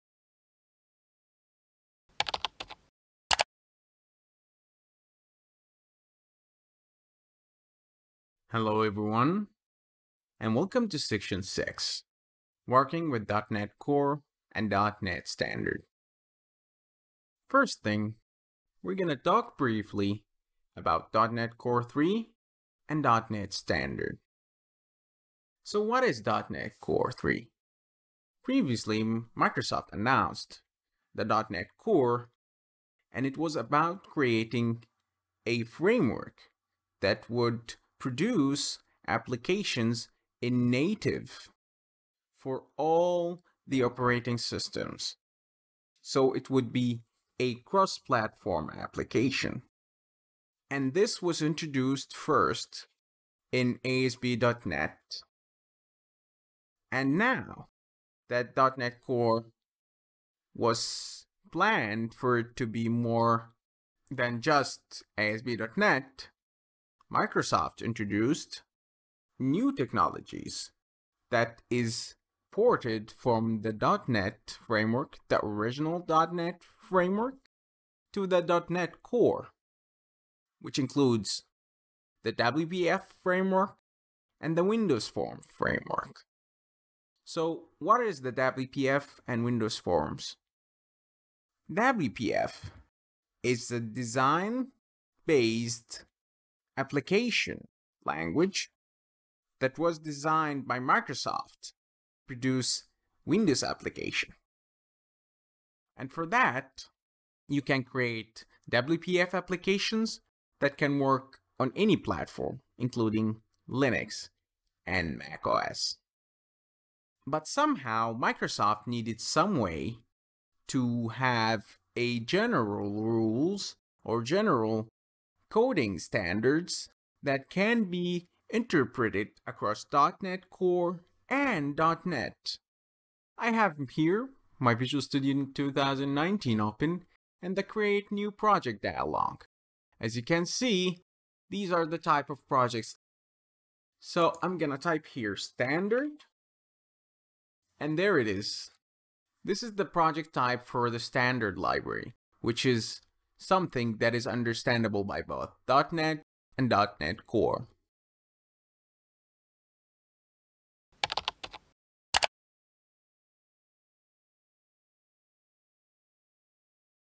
Learning C# 8 and .NET Core 3.0: Introduction to .NET Core 3 and .NET Standard 3 | packtpub.com
This video tutorial has been taken from Learning C# 8 and .NET Core 3.0. You can learn more and buy the full video course